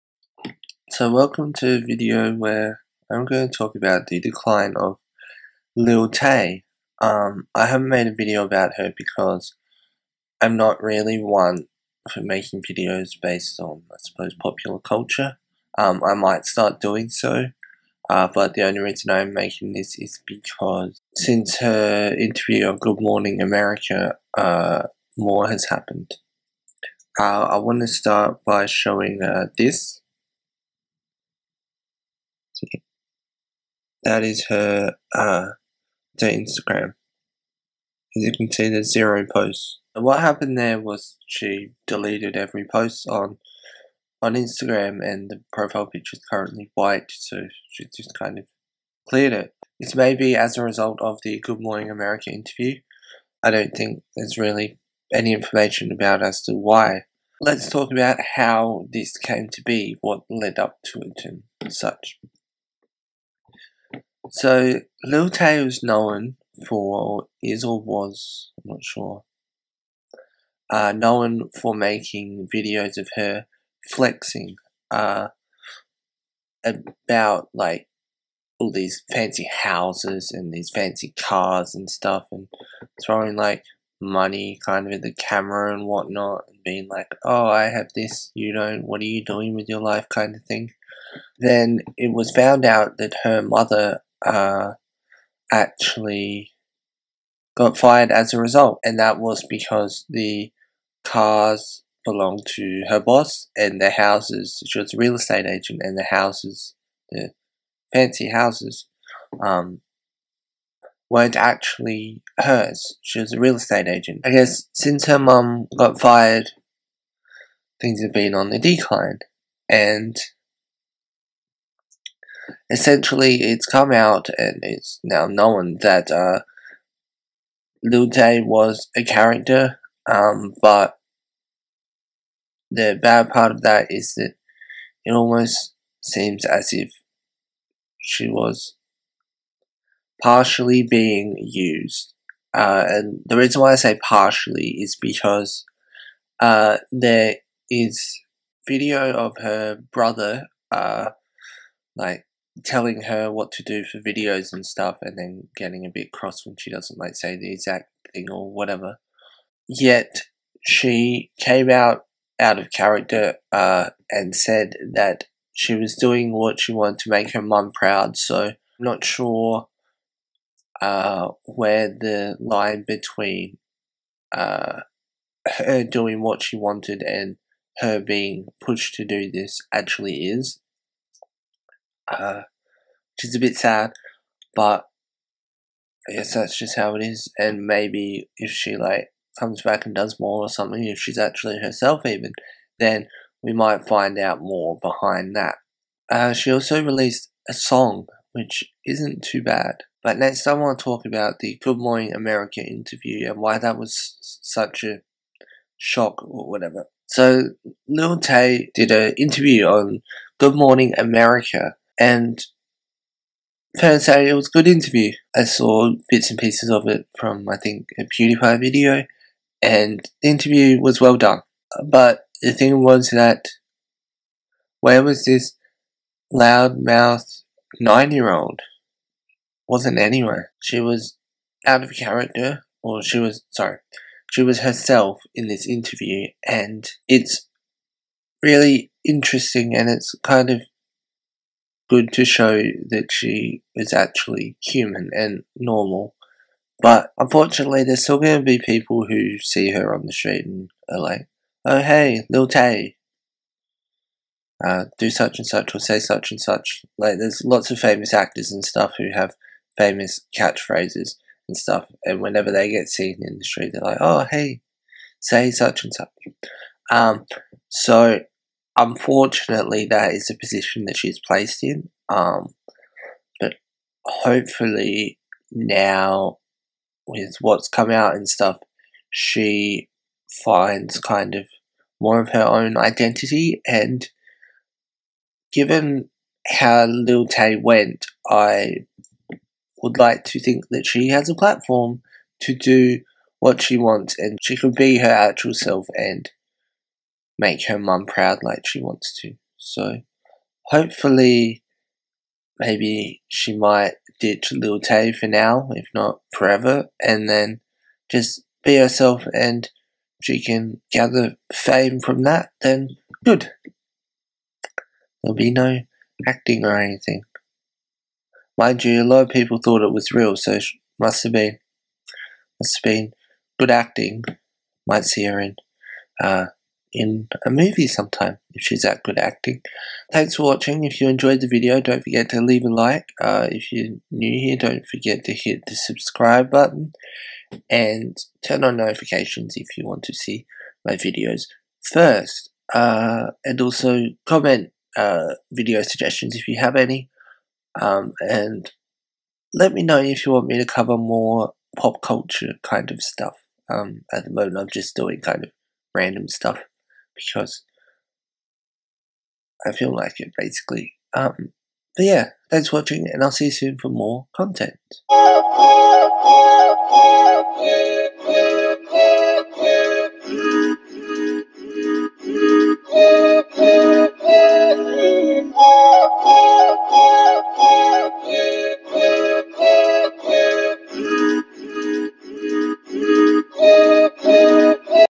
THE FALL OF LIL TAY
What led to lil tay removing every instagram post?
Apologises about the audio.

Accent? Australian